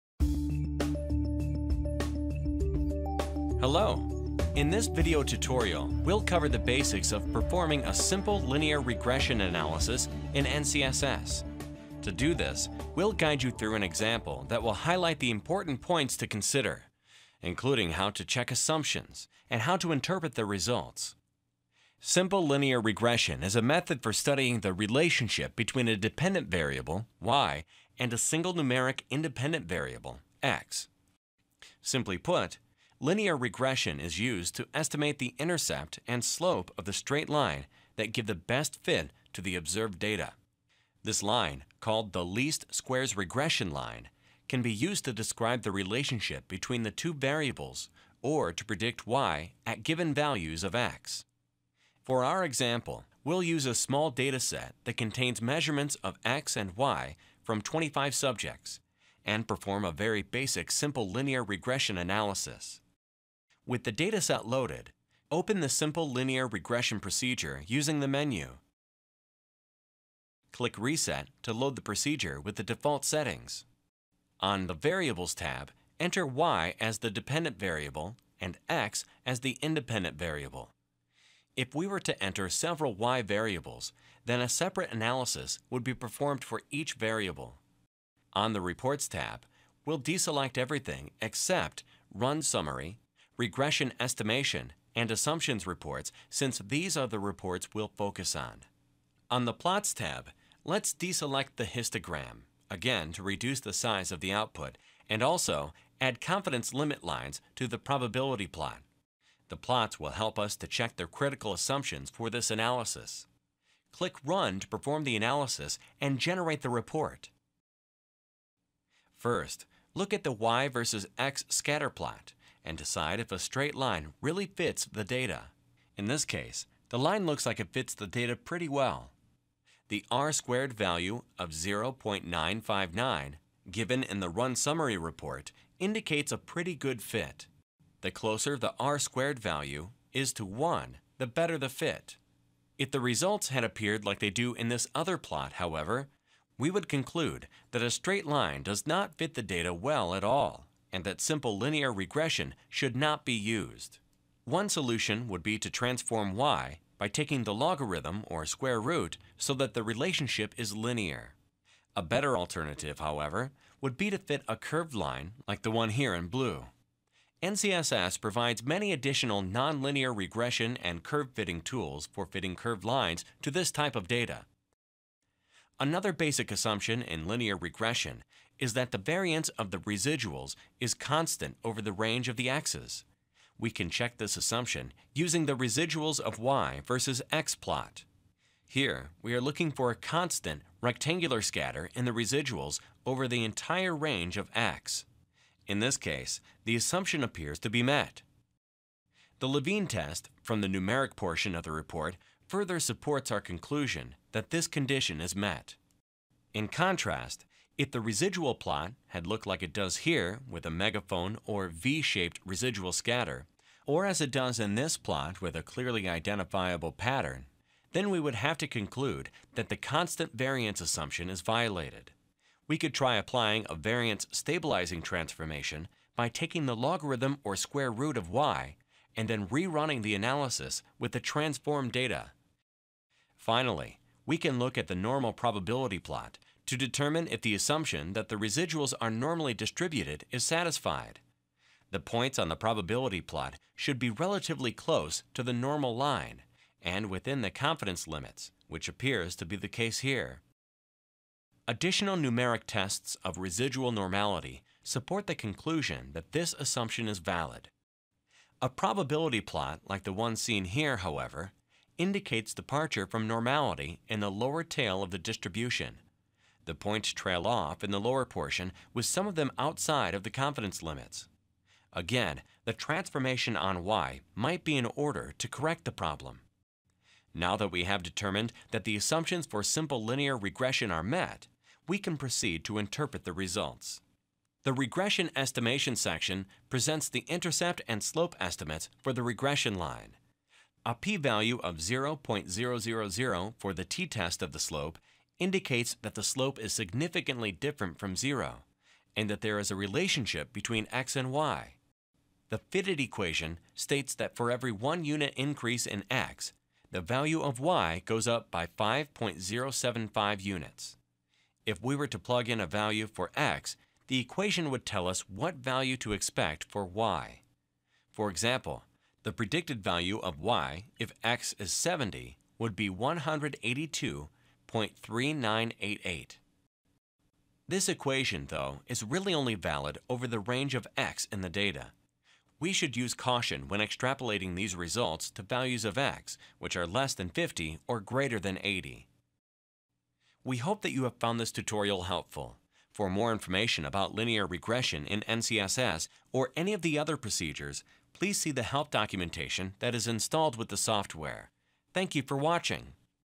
Simple Linear Regression in NCSS
In this video tutorial, we'll cover the basics of performing a Simple Linear Regression analysis in NCSS.
Simple Linear Regression is a method for studying the relationship between a dependent variable, Y, and a single numeric independent variable, X. Simply put, linear regression is used to estimate the intercept and slope of the straight line that give the best fit to the observed data. This line, called the least squares regression line, can be used to describe the relationship between the two variables or to predict Y at given values of X.
For our example, we'll use a small dataset that contains measurements of X and Y from 25 subjects and perform a very basic simple linear regression analysis.
With the dataset loaded, open the Simple Linear Regression procedure using the menu. Click Reset to load the procedure with the default settings.
On the variables tab, enter Y as the dependent variable and X as the independent variable. If we were to enter several Y variables, then a separate analysis would be performed for each variable.
On the reports tab, we'll deselect everything except the Run Summary, Regression Estimation, and Assumptions reports since these are the reports we'll focus on.
On the plots tab, let's deselect the histogram, again to reduce the size of the output, and also add confidence limit lines to the probability plot. The plots will help us to check the critical assumptions for this analysis.
Click Run to perform the analysis and generate the report.
First, look at the Y versus X scatterplot and decide if a straight line really fits the data. In this case, the line looks like it fits the data pretty well. The R-squared value of 0.959, given in the Run Summary Report, indicates a pretty good fit. The closer the R-squared value is to one, the better the fit.
If the results had appeared like they do in this other plot, however, we would conclude that a straight line does not fit the data well at all and that simple linear regression should not be used. One solution would be to transform Y by taking the logarithm or square root so that the relationship is linear. A better alternative, however, would be to fit a curved line, like the one here in blue. NCSS provides many additional nonlinear regression and curve fitting tools for fitting curved lines to this type of data.
Another basic assumption in linear regression is that the variance of the residuals is constant over the range of the X's. We can check this assumption using the Residuals of Y versus X plot. Here we are looking for a constant, rectangular scatter in the residuals over the entire range of X. In this case, the assumption appears to be met. The Levene test from the numeric portion of the report further supports our conclusion that this condition is met.
In contrast, if the residual plot had looked as it does here with a Megaphone- or V-shaped residual scatter, or as it does in this plot with a clearly identifiable pattern then we would have to conclude that the constant-variance assumption is violated. We could try applying a variance-stabilizing transformation by taking the logarithm or square root of Y and then re-running the analysis with the transformed data.
Finally, we can look at the Normal Probability Plot to determine if the assumption that the residuals are normally distributed is satisfied. The points on the probability plot should be relatively close to the normal line and within the confidence limits, which appears to be the case here. Additional numeric tests of residual normality support the conclusion that this assumption is valid.
A probability plot like the one seen here, however, indicates departure from normality in the lower tail of the distribution. The points trail off in the lower portion, with some of them outside of the confidence limits. Again, a transformation on Y might be in order to correct the problem.
Now that we have determined that the assumptions for simple linear regression are met, we can proceed to interpret the results.
The Regression Estimation Section presents the intercept and slope estimates for the regression line. A p-value of 0.000 for the t-test of the slope indicates that the slope is significantly different from zero and that there is a relationship between X and Y. The fitted equation states that for every one unit increase in X, the value of Y goes up by 5.075 units. If we were to plug in a value for X, the equation would tell us what value to expect for Y. For example, the predicted value of Y if X is 70 would be 182.3988.
This equation, though, is really only valid over the range of X in the data! We should use caution when extrapolating these results to values of X which are less than 50 or greater than 80.
We hope that you have found this tutorial helpful. For more information about Linear Regression in NCSS or any of the other procedures, please see the Help documentation that is installed with the software.